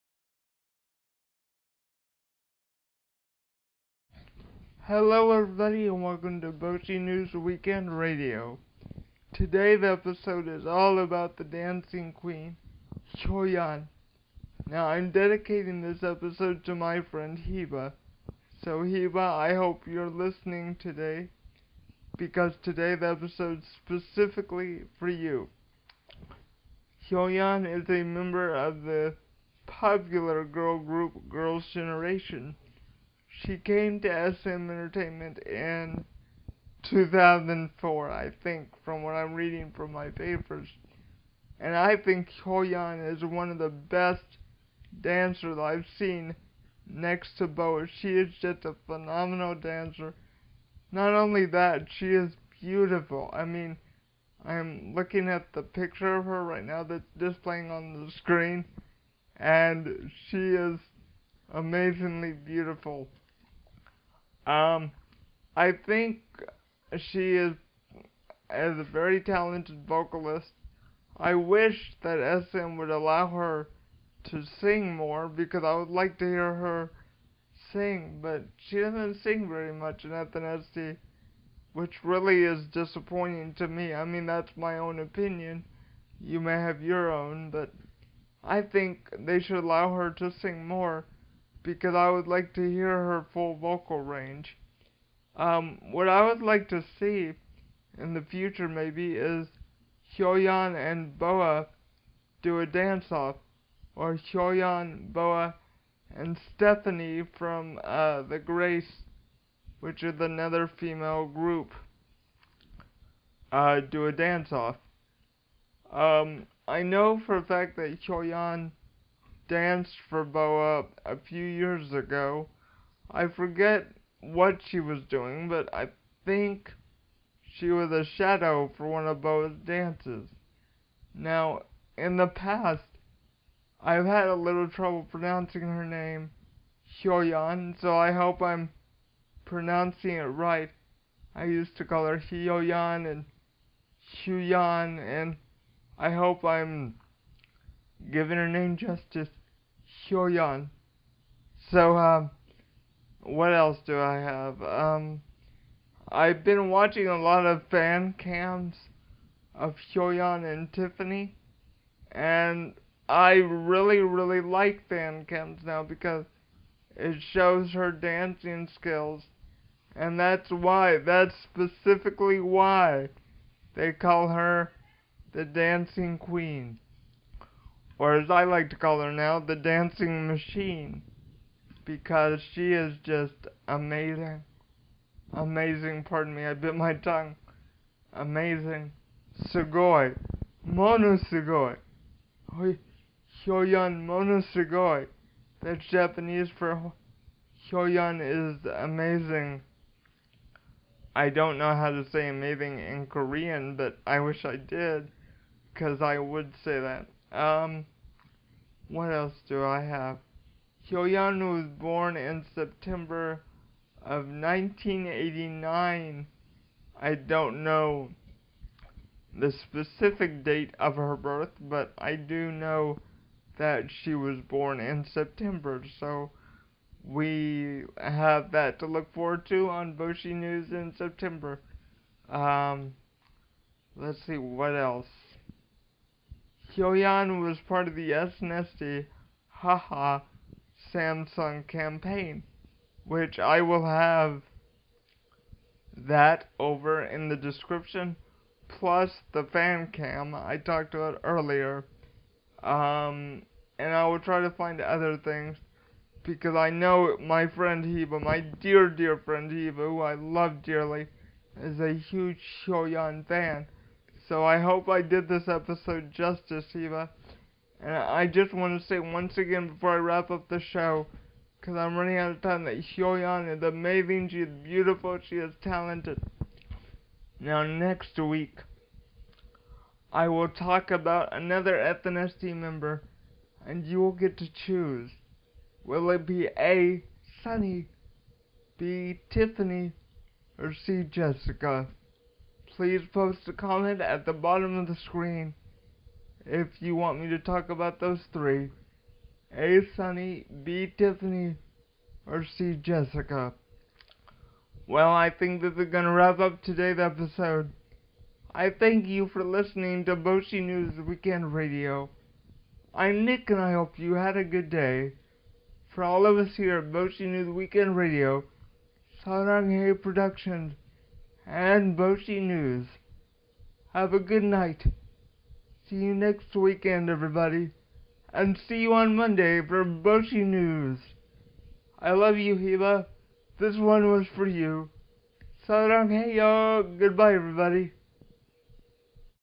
Boshi radio episode four
PLEASE READ FULL DESCRIPTION:
this week's episode is all about SNSD's dancing Queen Hyoyeon. I am dedicating this episode to my dear dear friend Heba, I hope that she likes this episode.